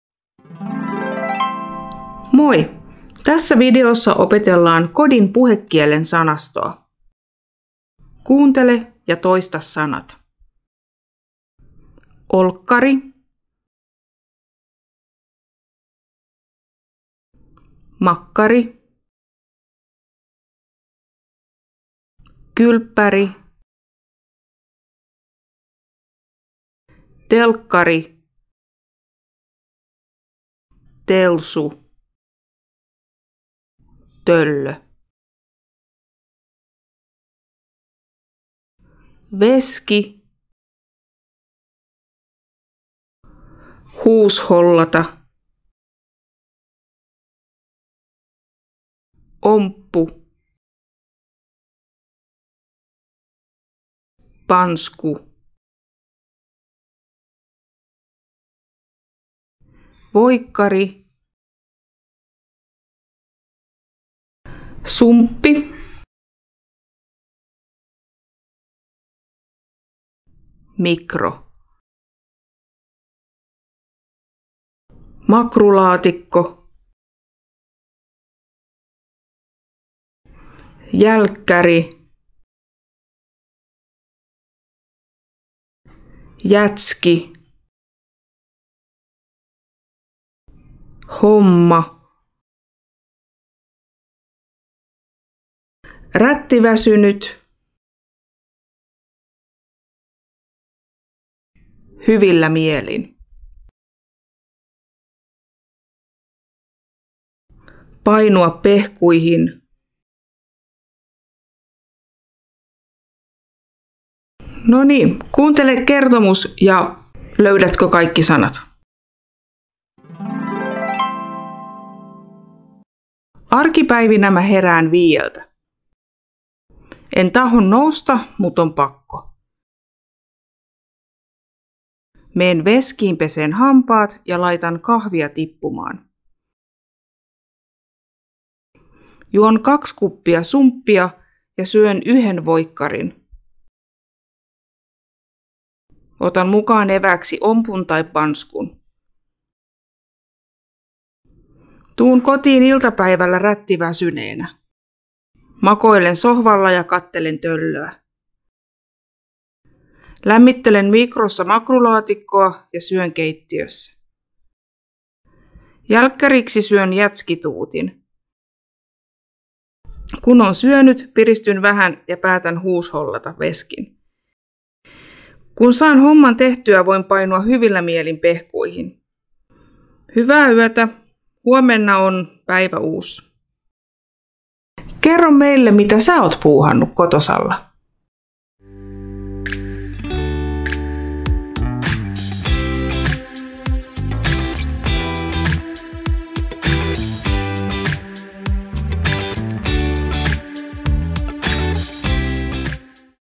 Kotosalla: kodin puhekieltä
Videossa on sanoja ja kertomus.
Kertomuksessa ei ole tekstiä. Se on kuunteluharjoitus. Voit laittaa tekstitykset päälle, jos haluat myös lukea tekstin.
Taso noin A2.1.